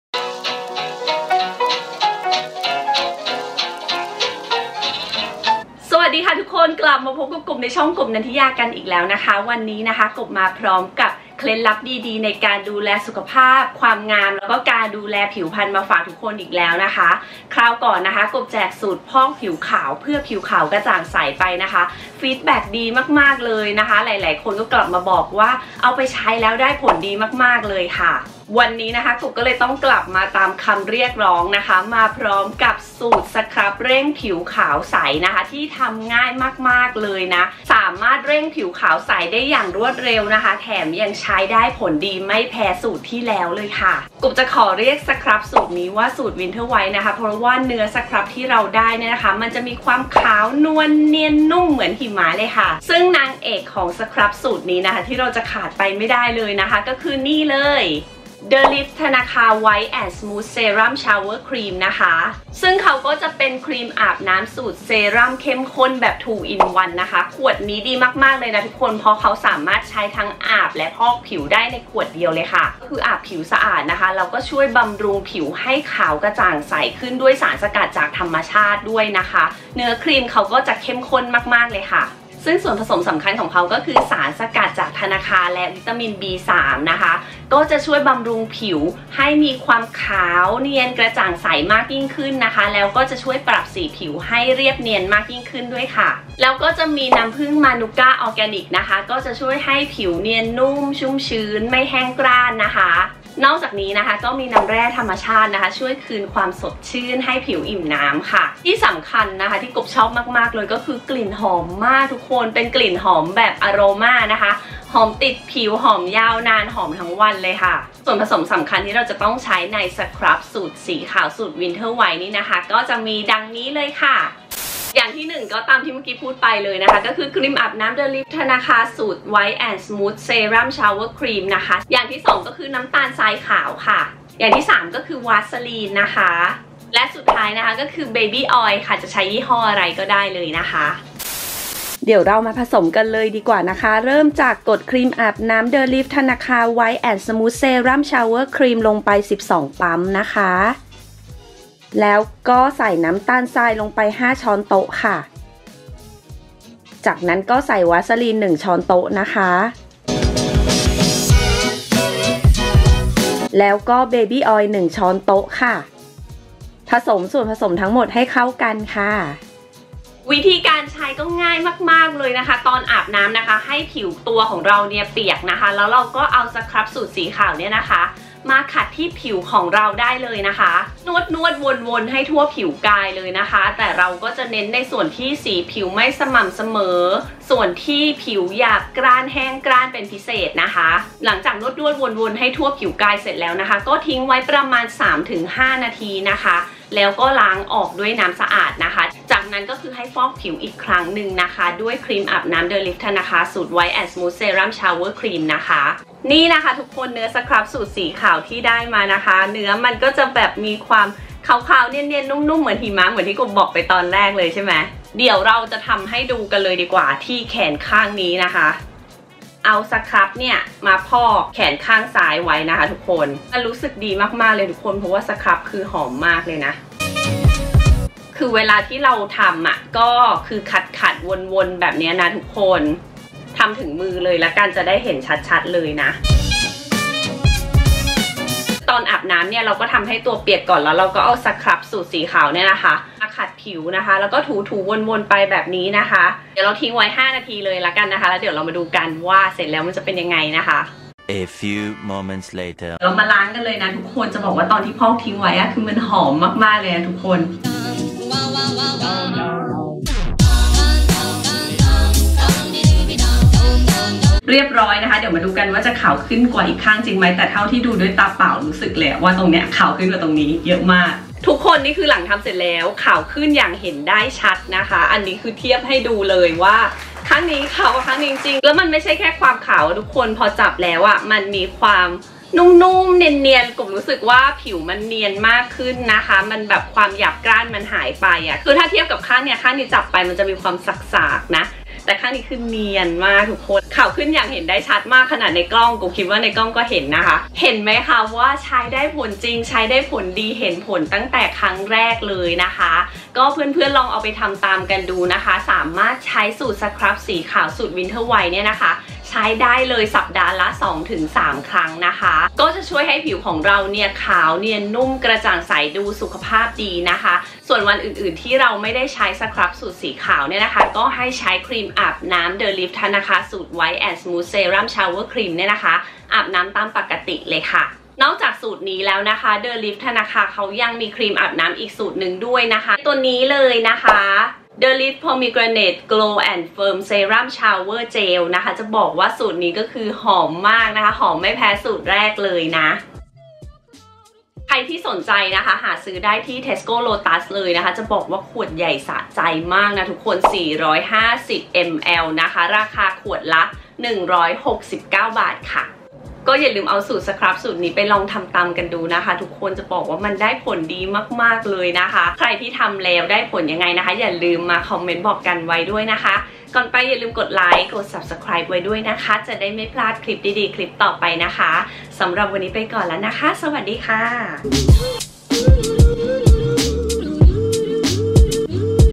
แจกสูตรสครับ DIY เร่งผิวขาวใส ขาวไวจริง ท้าให้ลอง I Kopnantiya
ฮัลโหล ทุกคน วันนี้กบมาแจกสูตร DIY สบู่พอกผิวสูตรเด็ด เพื่อผิวสวยใสเนียนนุ่มชุ่มชื้น ใครกำลังกลุ้มใจเรื่องผิวเสีย หมองคล้ำ สีผิวไม่สม่ำเสมอ ขอท้าให้ลองกันเลยค่ะ

ช่องทางในการติดตามอื่นๆ

คุณเจี๊ยบ (Manager)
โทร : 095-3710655